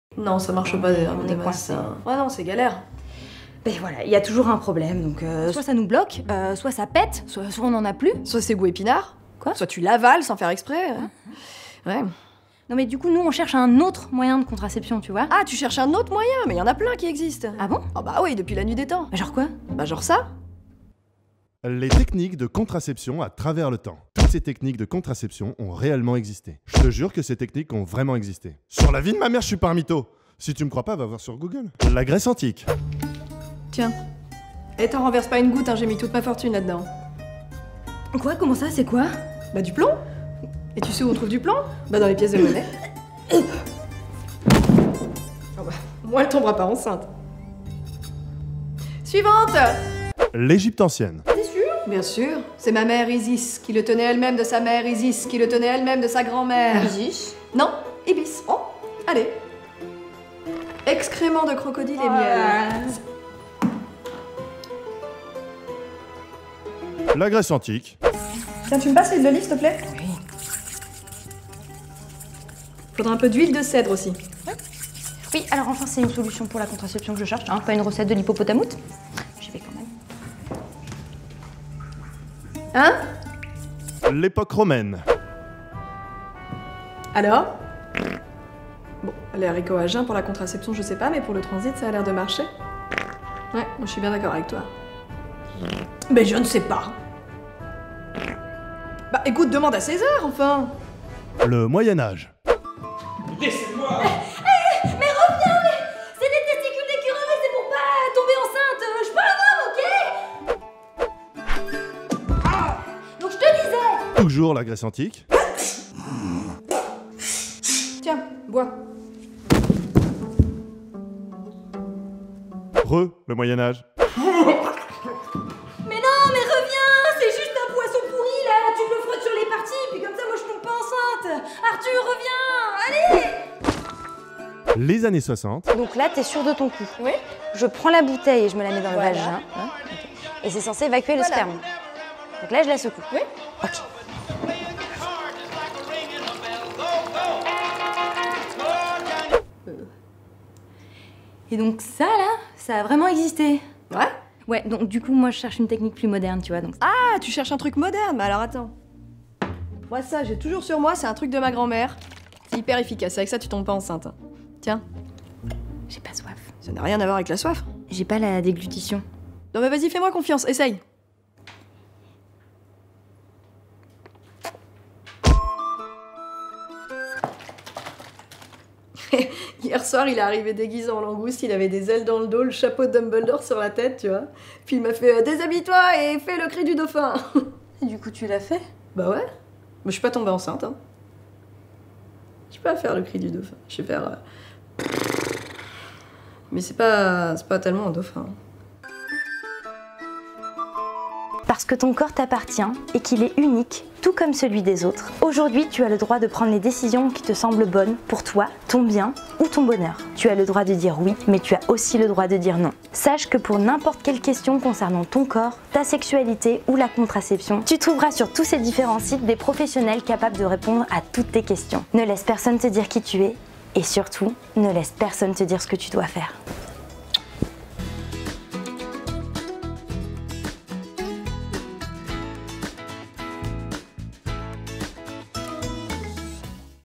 LÉA CAMILLERI - LA CONTRACEPTION
La contraception à travers les âges. Toutes ces techniques ont vraiment existé. Heureusement aujourd'hui il existe des techniques fiables pour prendre les décisions qui te semblent nécessaires ou bonnes pour toi et ton bonheur :D J'avais envie de parler ce sujet qui me tient a coeur et qui nous concerne tous.

Avec Léa Camilleri & Maud Bettina Marie
Réalisé par Vincent Scalera
Ecrit par Kaza Hachemi, Guillaume Gamand & Léa Camilleri

Chef Opérateur :  Camille Salvan
Ingénieur son : Charles Grégoire
Montage : Antoine McFly
Etalonnage : Antoine Guibert
Graphisme : Ando Raminoson & Théo Meunier
Directrice de production : Mélina TATOPOULOS
1ère ASSISTANTE/ SCRIPTE : Anne JAMMET
COSTUMIERE : Anna SNURAWA
MAKE UP : Romane De Luca
COIFFURE : Karmen Sefer
REGISSEUR : Franck Bourdin

Merci à Webedia, Anne Duval & Frank Mickael